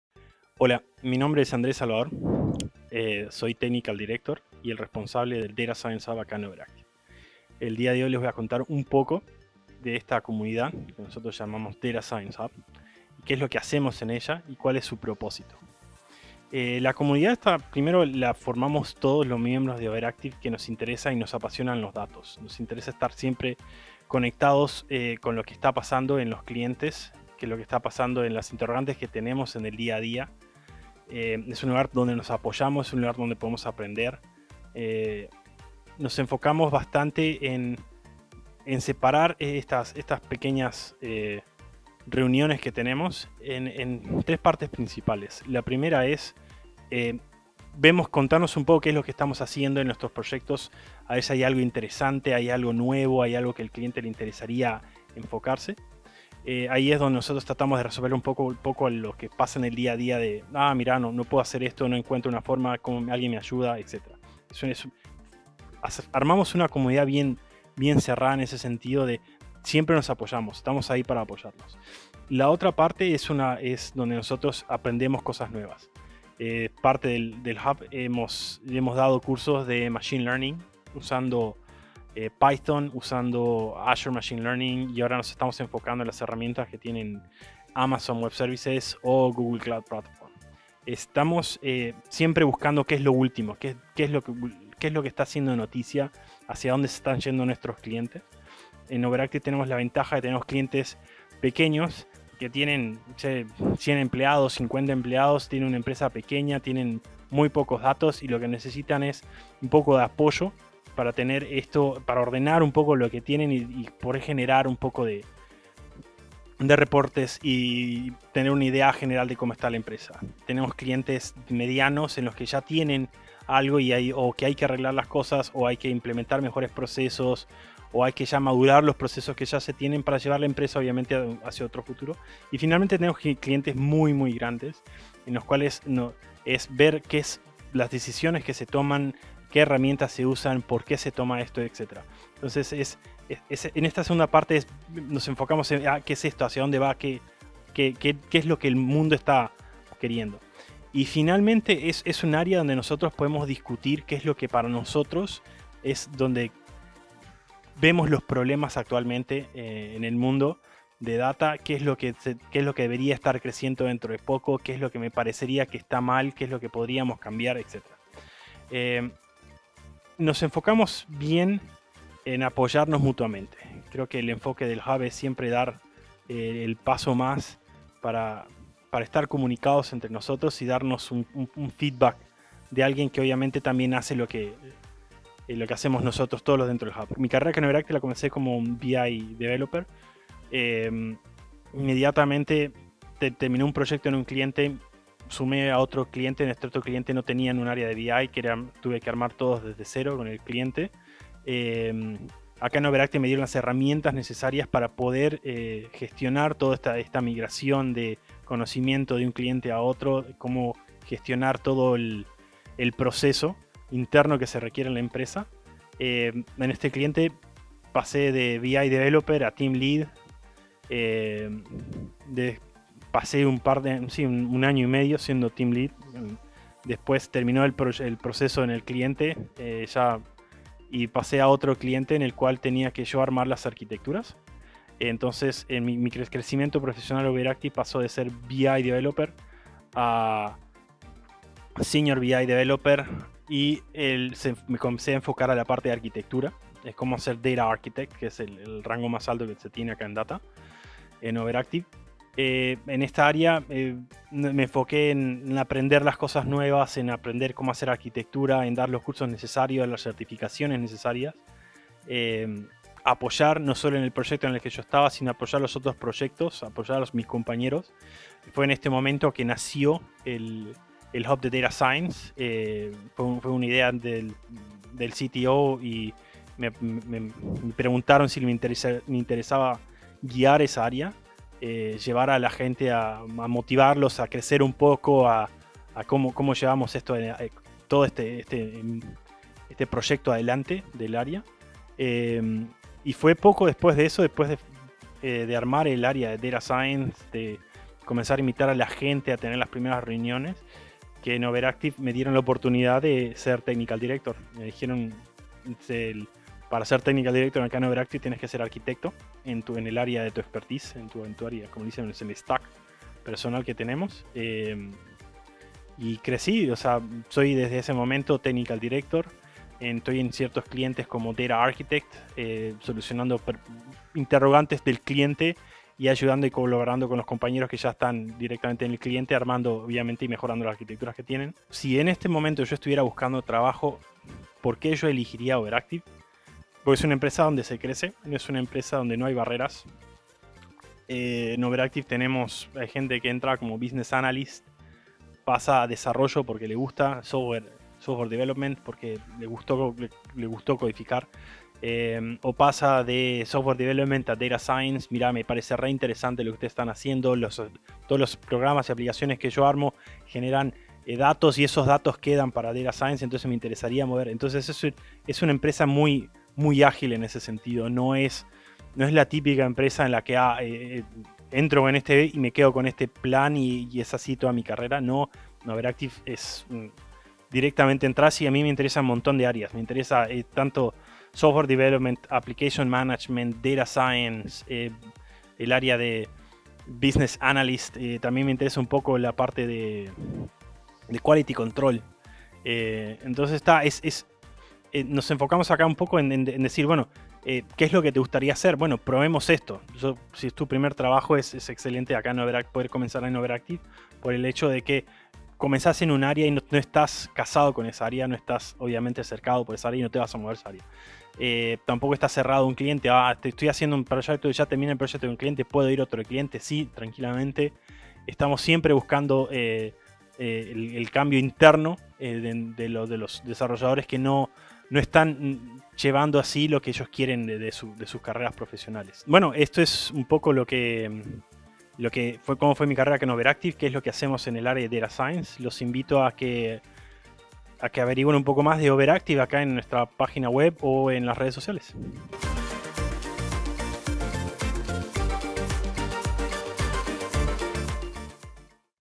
Data Science in OA
Conferencias dictada por Overactive en el marco de la Feria de Empleo de la Facultad de Ingeniería, realizada el 20 de octubre de 2020.